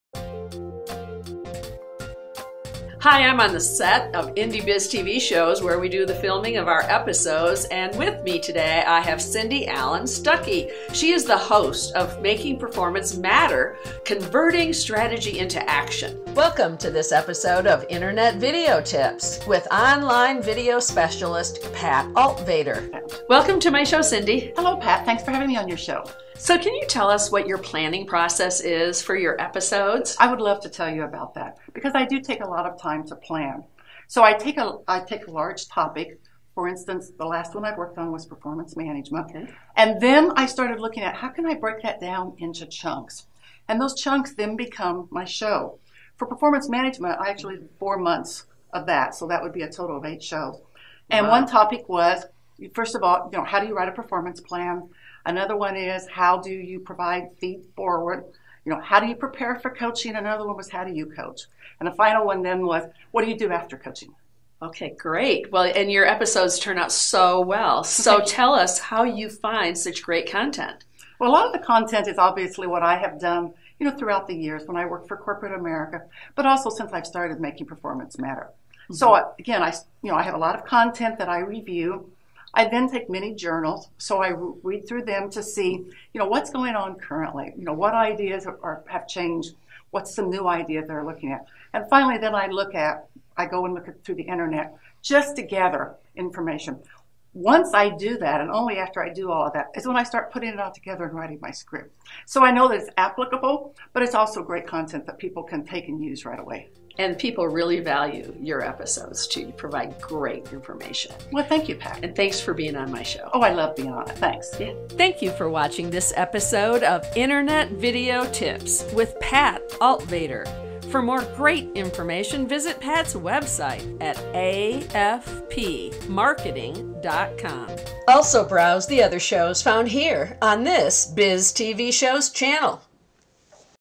Pat Altvater Shares Video Best Practices
Pat Altvater, in her video best practices series, interviews Cindy Allen-Stuckey to find out how she researches her topics for her videos